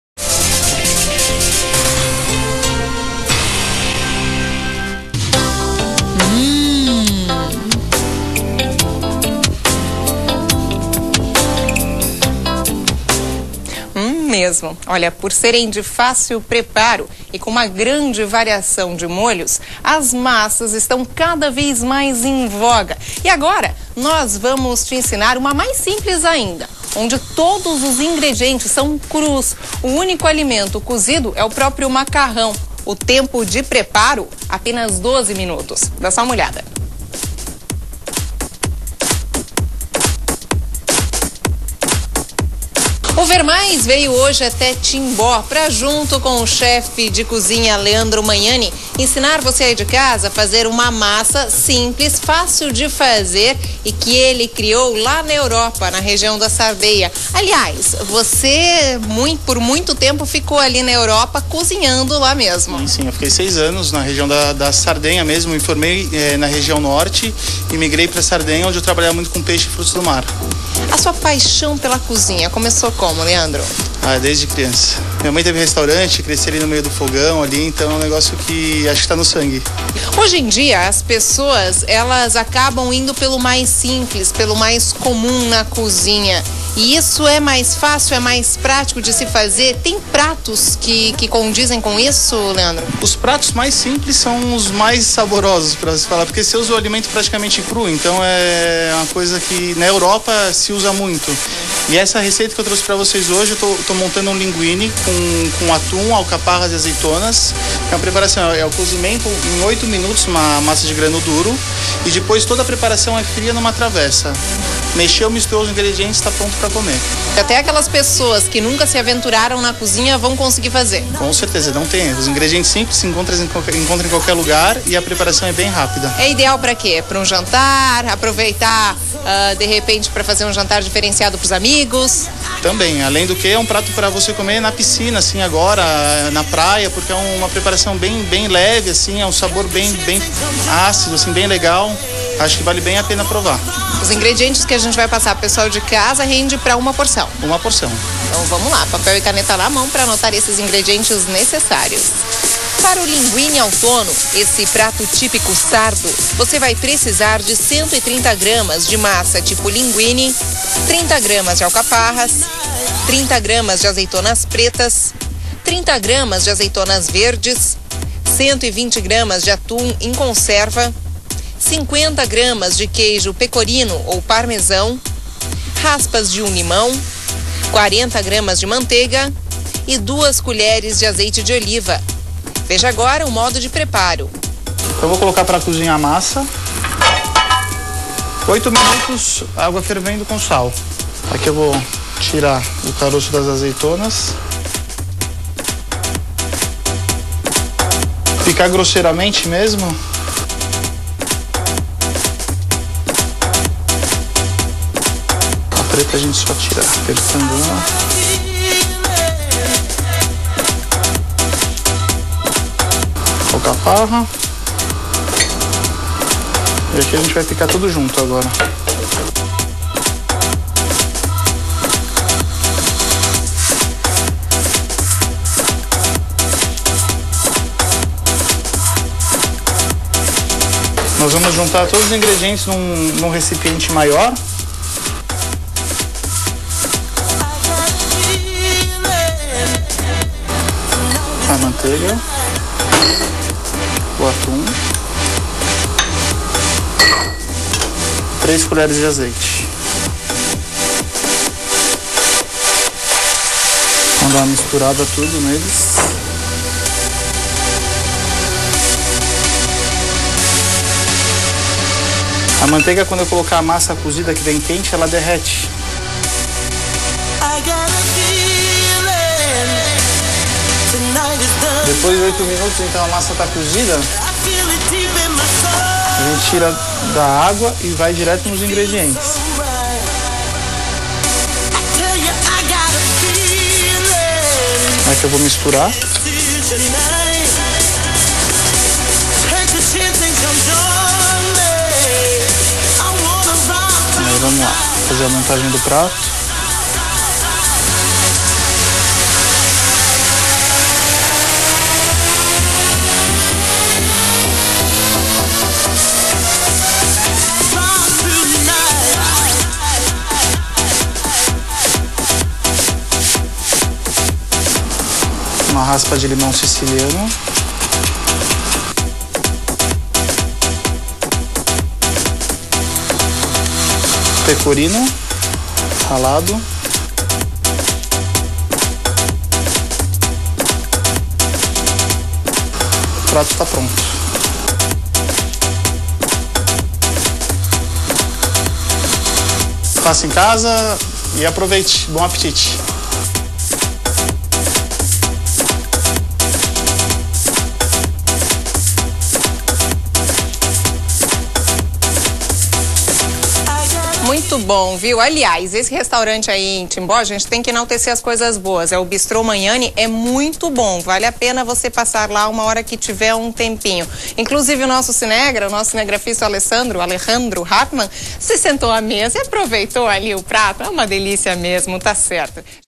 Macarrão pronto em 12 mimutos
Macarrão com atum e azeitonas fica pronto em 12 mimutos